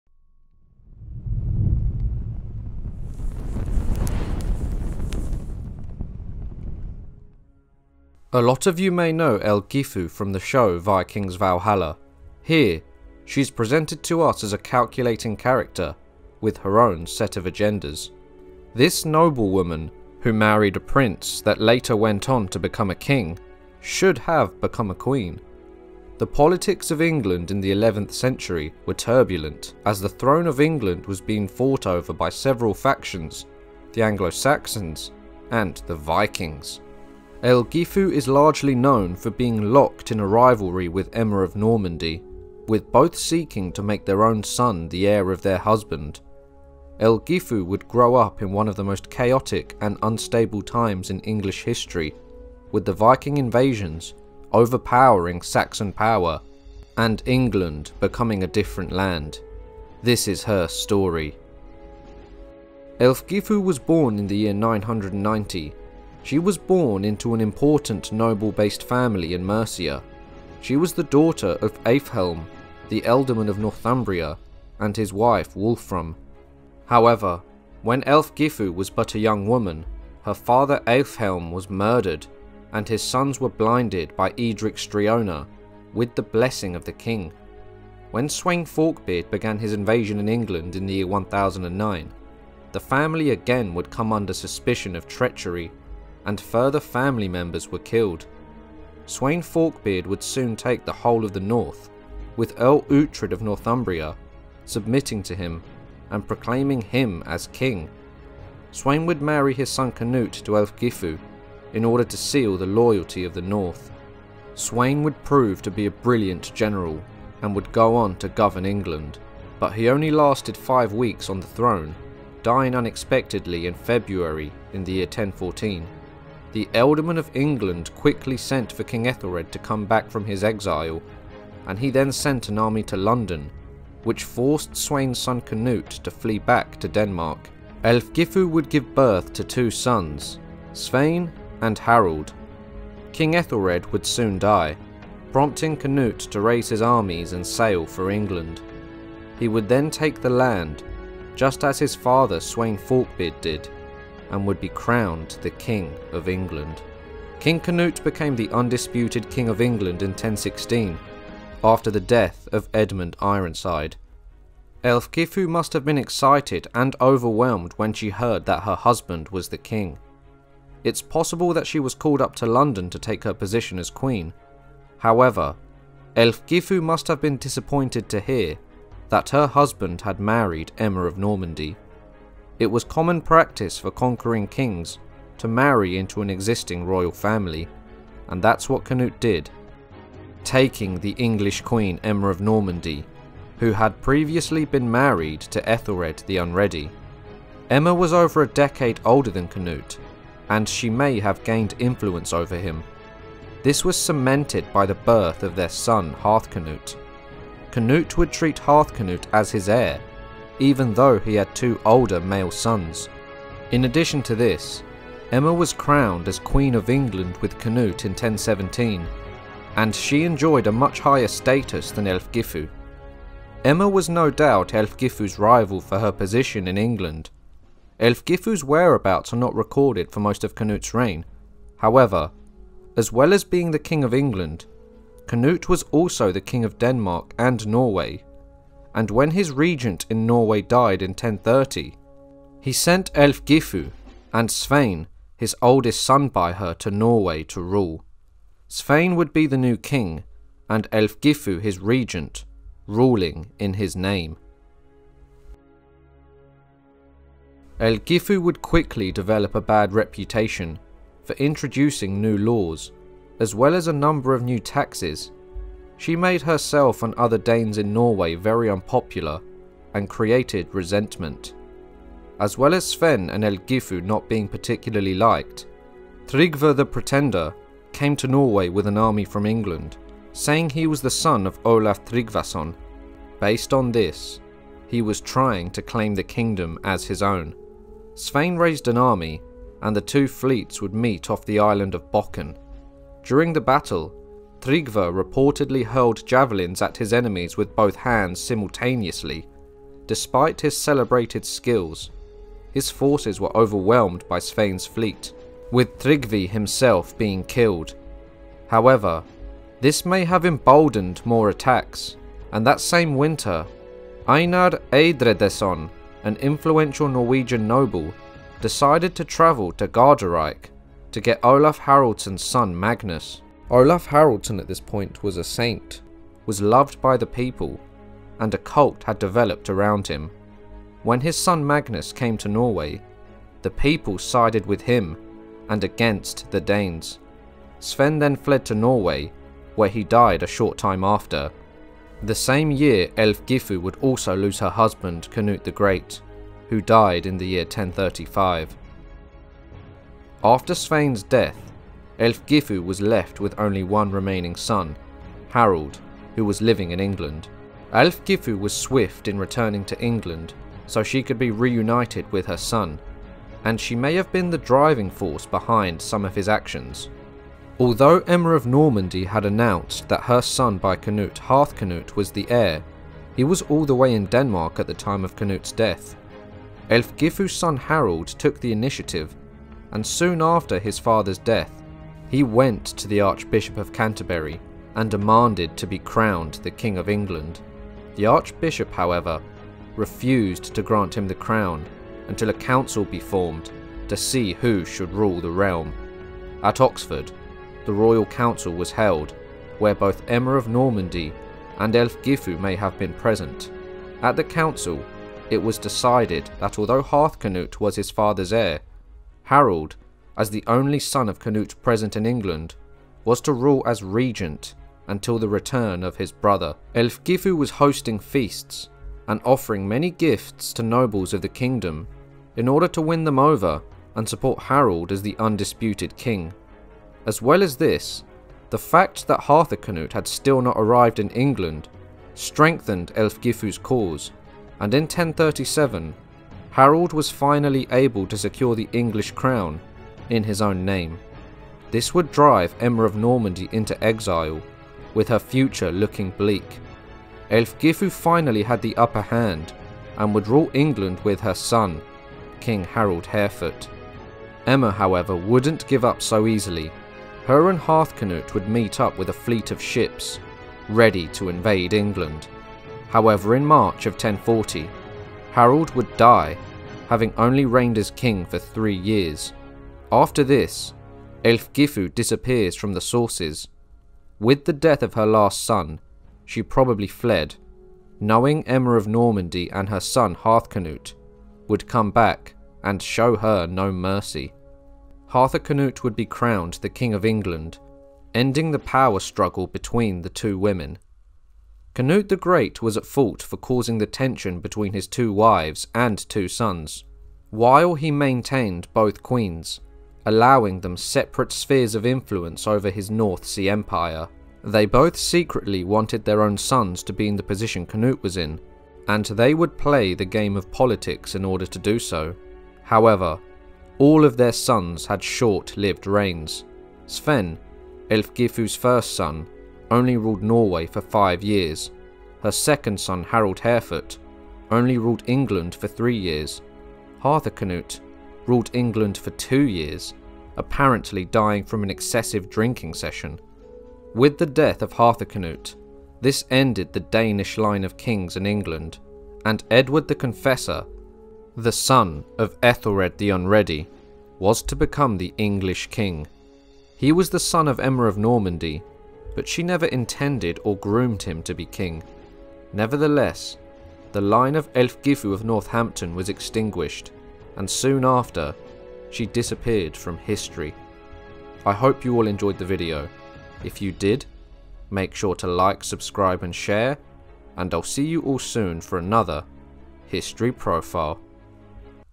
The Real Life of Ælfgifu of Northampton | Vikings Valhalla
A lot of you may knowÆlfgifu from the show Vikings Valhalla. Here she is presented to us as a calculating character with her own set of agendas. However, what was her life like according to the sources? This noblewoman who married a Prince who later goes on to become a King should have become a queen. The politics of England in the 11th century were turbulent as the throne of England was being fought over by several factions, the Anglo Saxons and the Vikings.

Ælfgifu is largely known for being locked in a rivalry with Emma of Normandy, with both seeking to make their own son the heir of their husband and both to extreme lengths of doing so. Ælfgifu would grow up in one of the most chaotic and unstable times in English history with the Viking invasions overpowering Saxon power and England becoming a different land. This is her story.

Chapters:
00:00 Introduction
01:02 Chapter 1 - Early Life & War
02:38 Chapter 2 - A Tense Rivalry
04:16 Chapter 3 - The death of Kings
05:50 Chapter 4 - The Succession after Cnut

All Credit for the Music goes to: Savfk - Music